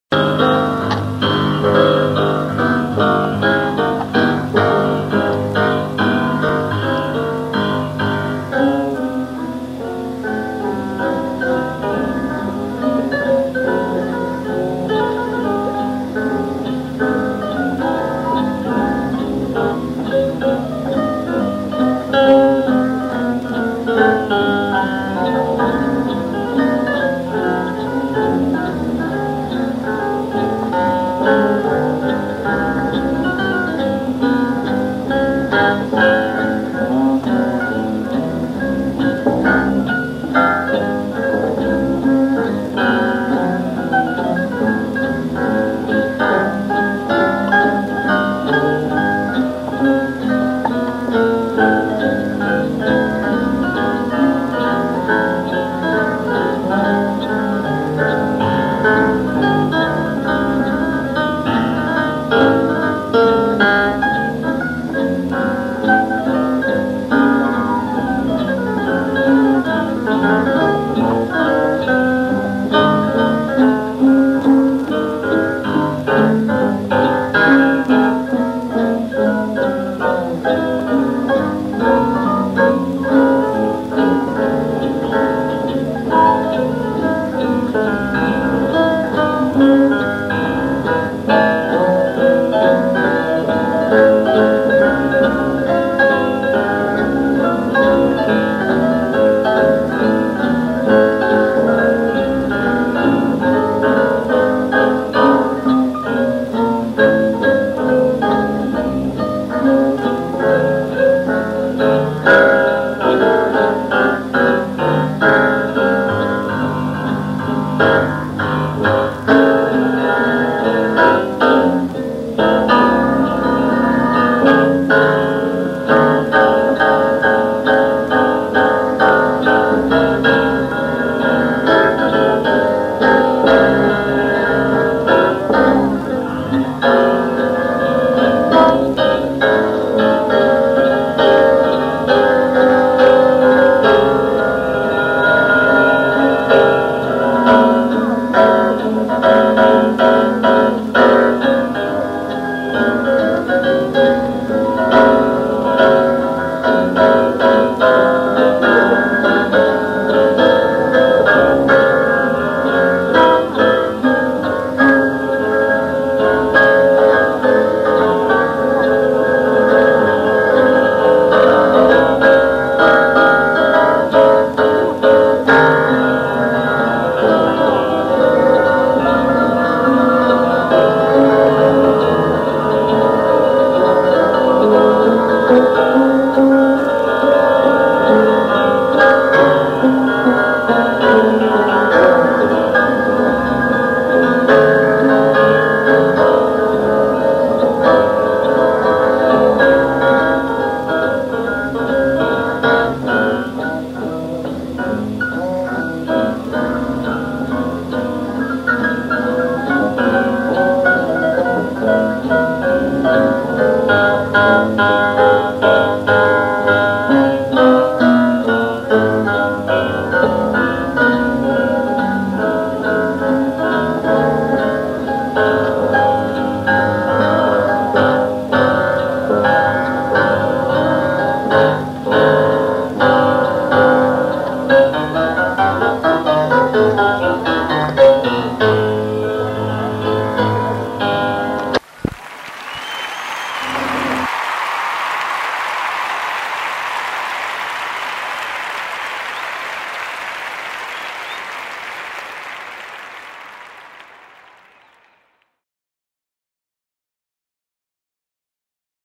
NAT PIERCE & ONE O'CLOCK JUMPERS PESCARA JAZZ 1987
PESCARA-LE NAIADI-26 LUGLIO 1987
XV FESTIVAL INTERNAZ.DEL JAZZ DI PESCARA-1987
ONE O'CLOCK JUMPERS